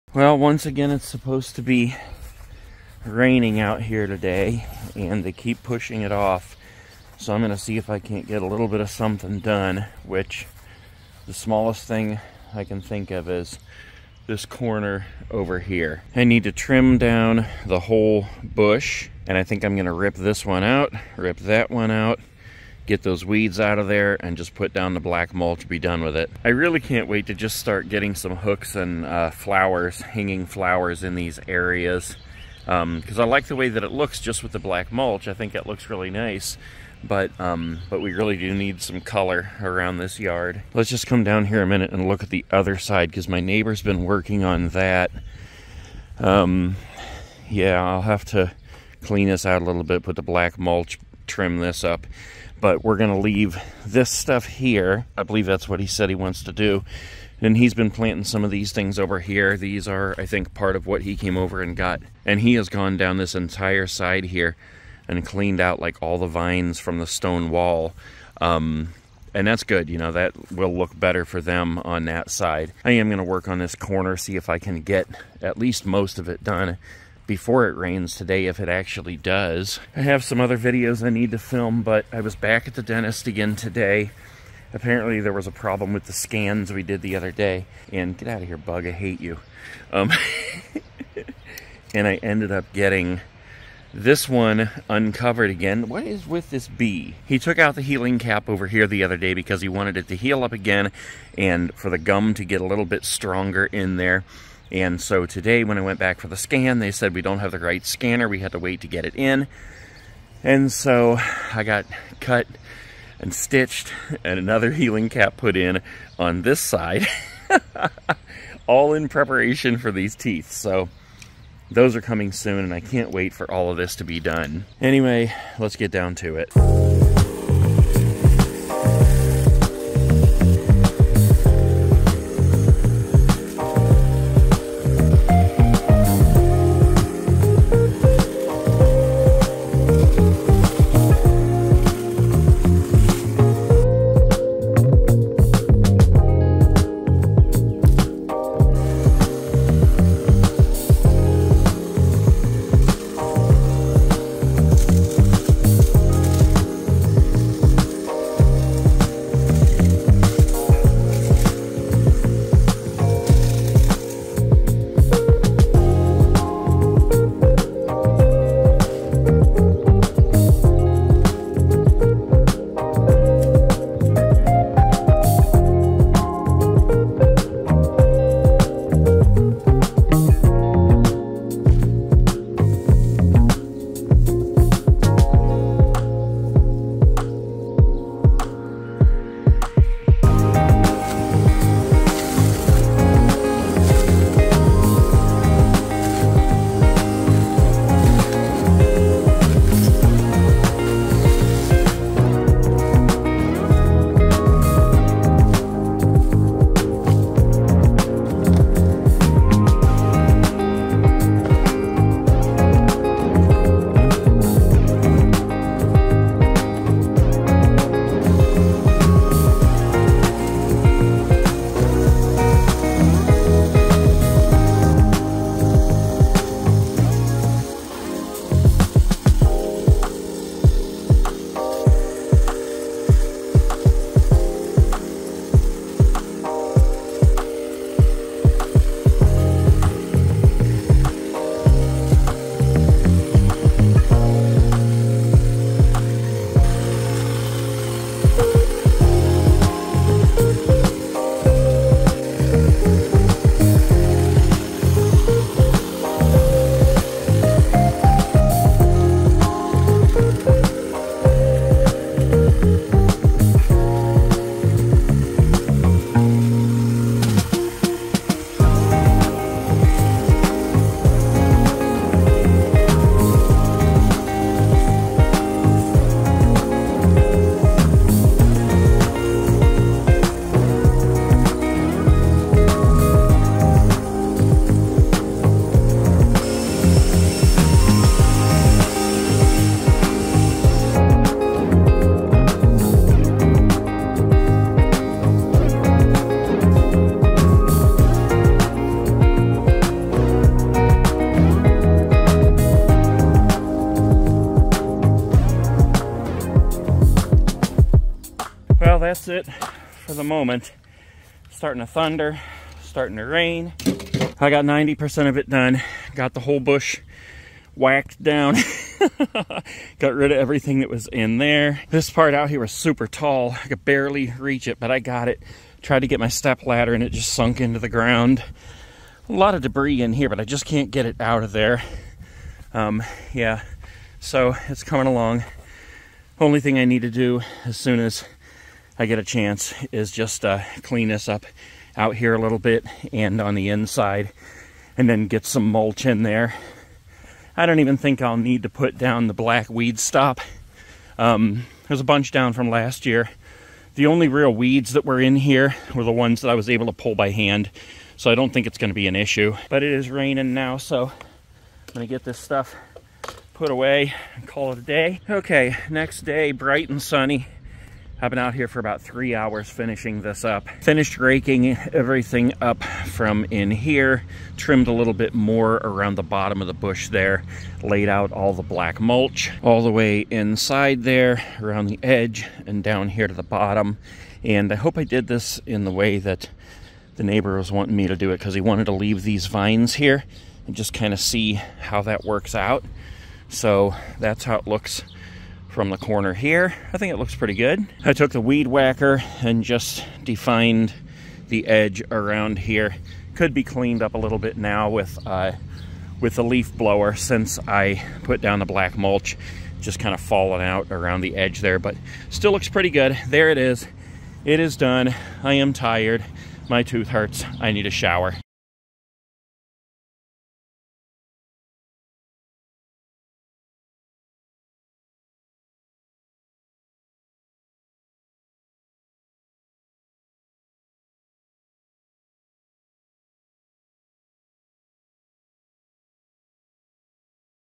Landscaping around large bushes With Vigoro Black Mulch from Home Depot
Today I am landscaping the front corner bushes in my front yard, cutting out a couple of old small shrubs planted by the former home owner, trimming the hedges, and laying down Vigoro black mulch from Home depot.